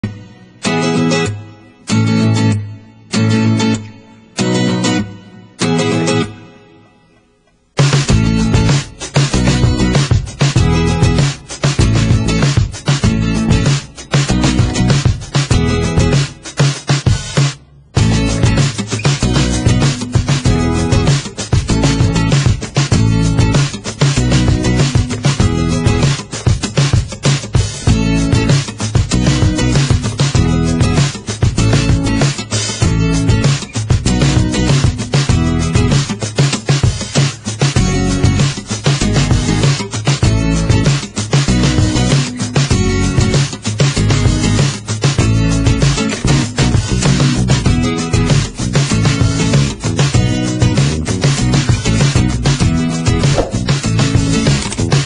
WA 082231777788 - Bagaimana Cara Pakai Krim Hn Original Lihat Disini Ini Cara Pakainya
Cream hn original adalah cream perawatan wajah yang awal mula dibuat oleh Hetty Nugrahati, Cream ini begitu aman dipakai karena sudah terdapat ijin dari BPOM, kemudian komposisinya sedikitpun tanpa menggunakan bahan kimia. Cream hn memiliki banyak khasiat antara lain mencegah kulit kusam, menuntaskan jerawat, melembabkan kulit dan lain-lain.